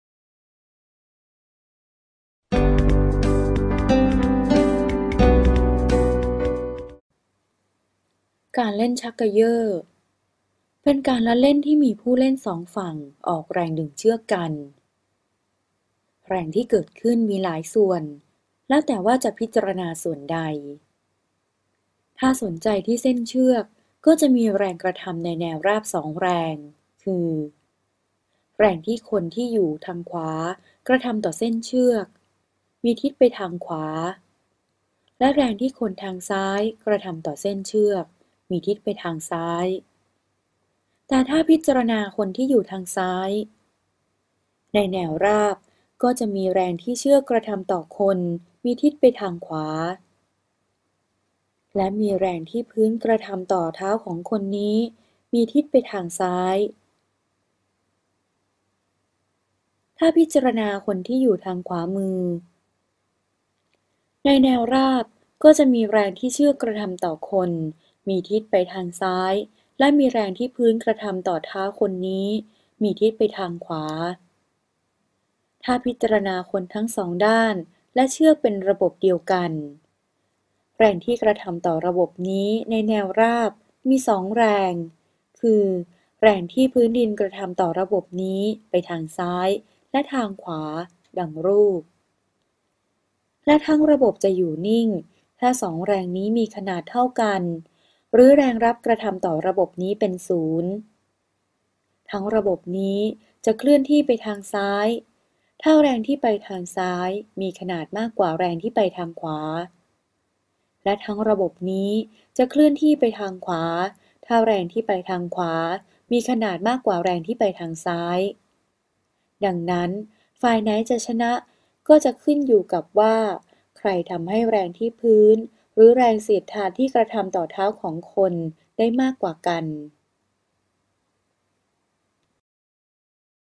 การชักเย่อ วิทยาศาสตร์ ม.4-6 (ฟิสิกส์)
การชักเย่อ
ผลิตโดย สสวท.
สื่อชิ้นนี้ประกอบการใช้งานกับ "หนังสือเรียนรายวิชาพื้นฐาน วิทยาศาสตร์ ม.4-6 การเคลื่อนที่และแรงในธรรมชาติ"
วิทยาศาสตร์ ม.4-6 (ฟิสิกส์)